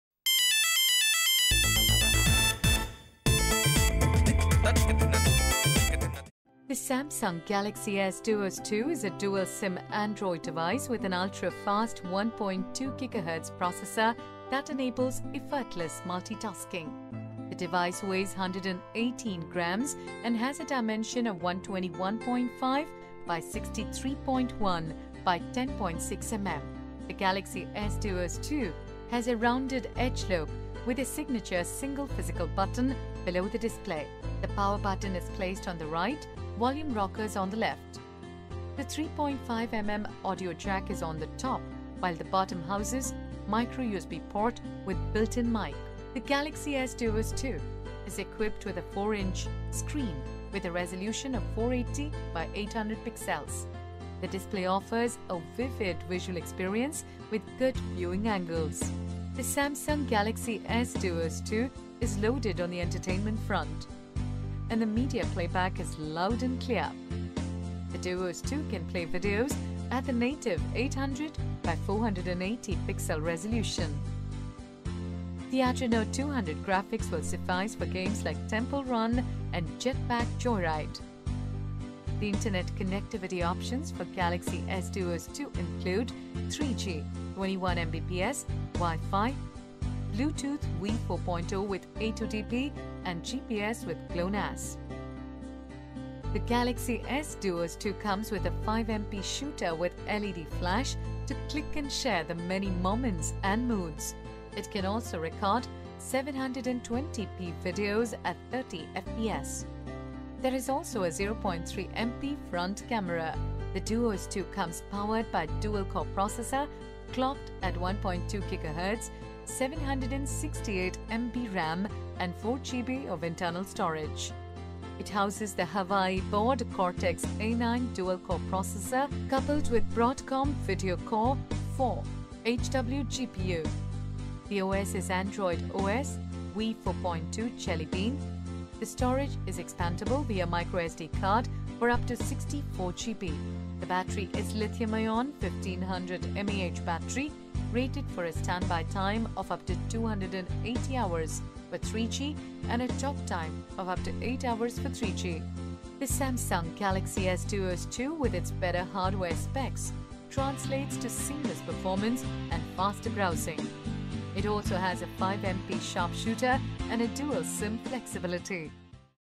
Samsung S 7582 Galaxy S Duos II - UniverCell The Mobileexpert Reviews
The Samsung Galaxy S Duos 2 is a dual-SIM Android device with an ultra fast 1.2Ghz processor that enables effortless multi-tasking.

The device weighs 118g and has a dimension of 121.5 x 63.1 x 10.6 mm. The Galaxy S Duos 2 has a rounded edge look with a signature single physical button below the display. The power button is placed on right, volume rockers on left. The 3.5mm audio jack is on the top while the bottom houses micro USB port with built in mike.
The Galaxy S Duos 2 is equipped with a 4 "screen with a resolution of 480 x 800 pixels. The display offers a vivid visual experience with good viewing angles.

The Samsung Galaxy S Duos 2 is loaded on the entertainment front. It has FM radio with RDS support and the media playback is loud and clear. The Duos 2 can play videos at the native 800 x 480-pixel resolution. The Adreno 200 graphics will suffice for games like Temple Run and Jetpack Joyride.

The internet connectivity options for Galaxy S Duos 2 include 3G (21Mbps), Wi-Fi, Bluetooth v4.0 with A2DP and GPS with GLONASS.

The Galaxy S Duos 2 comes with a 5MP shooter with LED flash to click and share the many moments and moods! It can also record 720p videos at 30fps. There is also a 0.3MP front camera.

The Duos 2 comes powered by dual core processor clocked at 1.2GHz, 768 MB RAM and 4GB of internal storage. It houses the Hawaii board Cortex A9 dual core processor coupled with Broadcom VideoCore IV HW GPU. The OS is Android OS, v4.2 (Jelly Bean). The storage is expandable via microSD card for up to 64GB.

The battery is Li-Ion 1500 mAh battery rated for a stand-by time of up to280 h for 3G and a talk time of up to 8 h for 3G.

The Samsung Galaxy S Duos 2 with its better hardware specs translates to seamless performance and faster browsing. It also has a 5MP sharp shooter and a dual SIM flexibility.